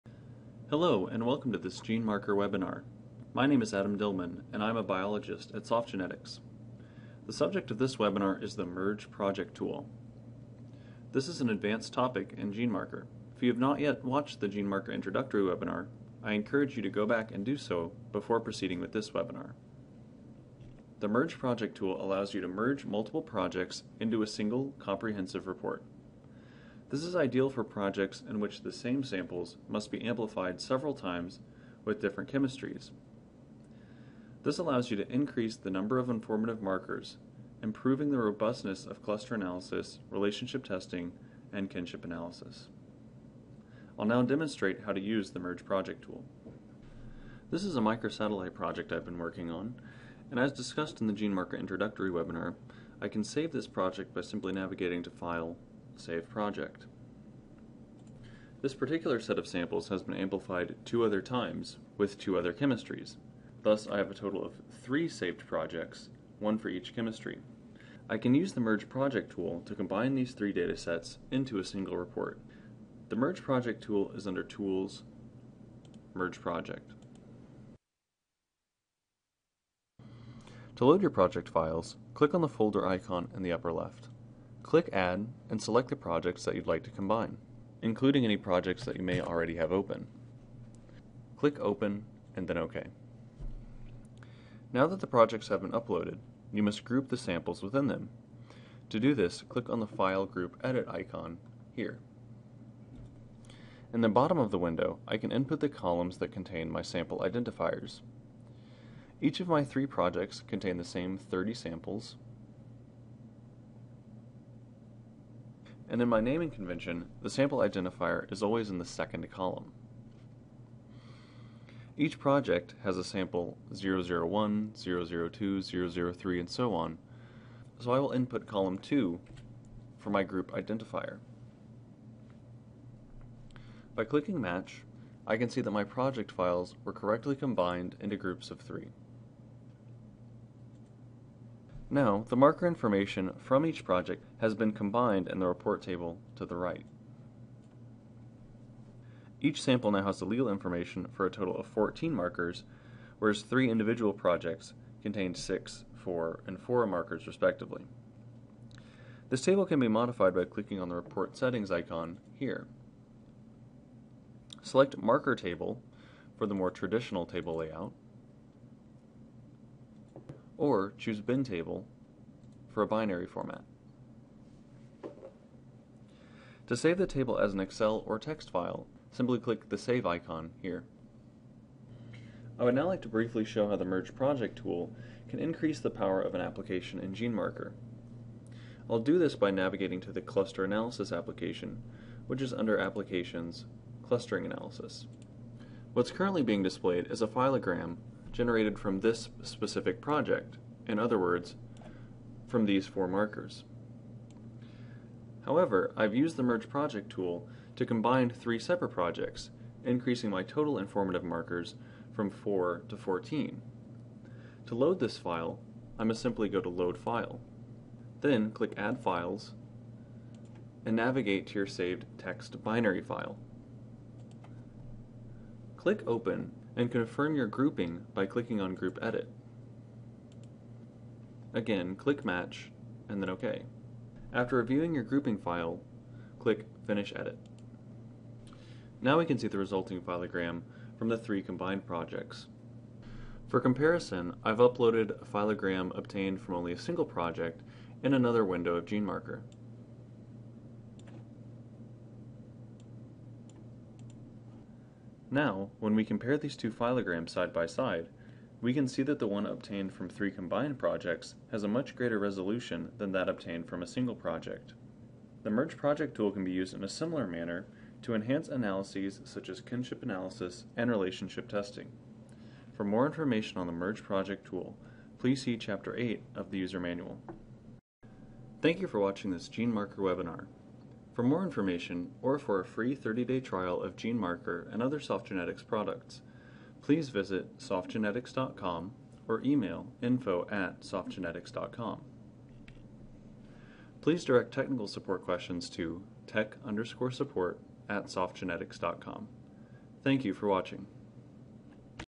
Merge Project Tool
This webinar discusses a tool in GeneMarker called the merge project tool. This tool enables users to combine multiple projects into a single, comprehensive report. This is especially useful for increasing the number of informative markers in a project, therby improving the strength of analyses such as relationship testing and cluster analysis.